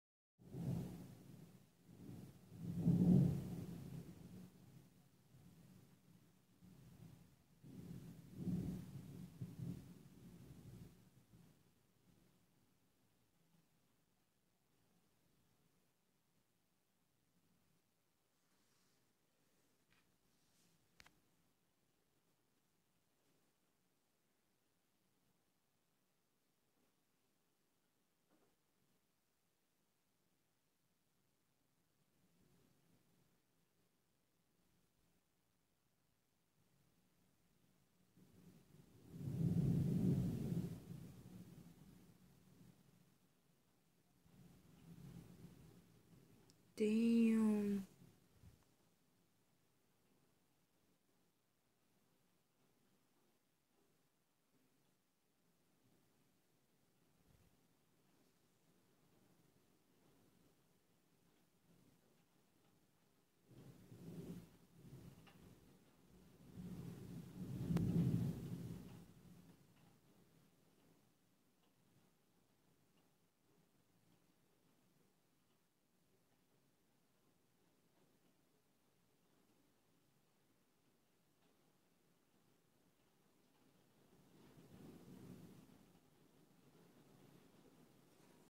Lightning and thunder⚡☁☔⚠⚠
This a supercell storm in Romania,it's really dangerous it's like a whole typhun surrounded by creepy looking clouds,if you enjoyed this thunder and lightning strike go and leave a like and maybe subscribe and turn on my post notifications

@_emo__voador_ on Instagram